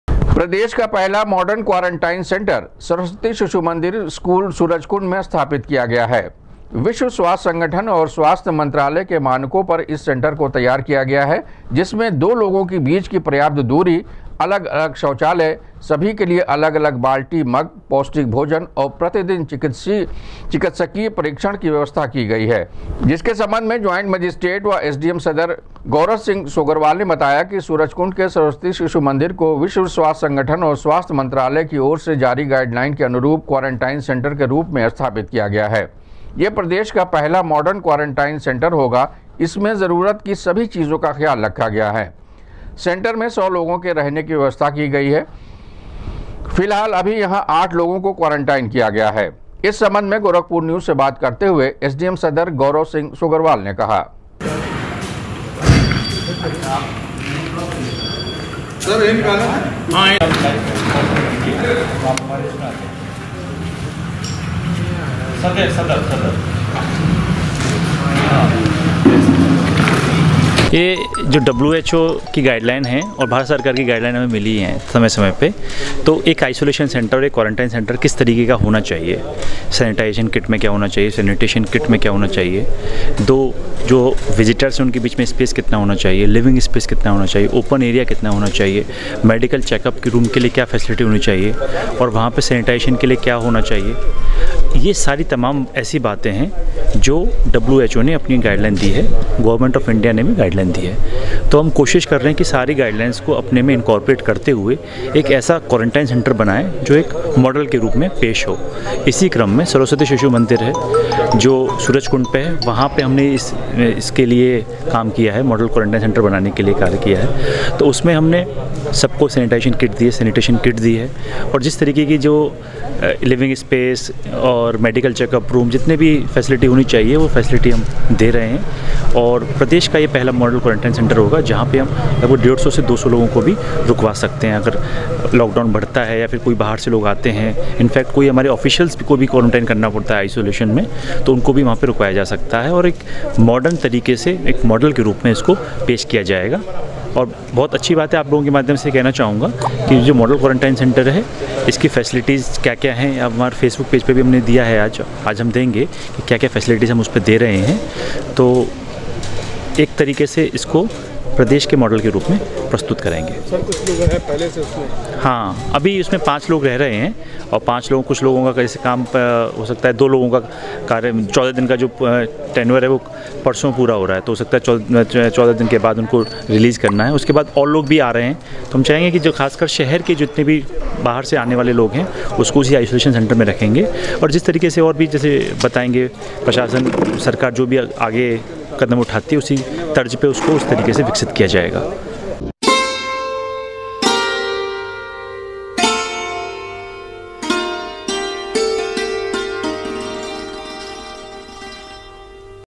सरस्वती शिशु मंदिर स्कूल सूरजकुंड में बनाया गया प्रदेश का पहला मॉडर्न क्वारंटाइन सेंटर,100
सरस्वती शिशु मंदिर स्कूल सूरजकुंड में बनाया गया प्रदेश का पहला मॉडर्न क्वारंटाइन सेंटर,100 लोगों के रहने की है व्यवस्था,सोशल डिस्टेंसिंग का रखा गया है ध्यान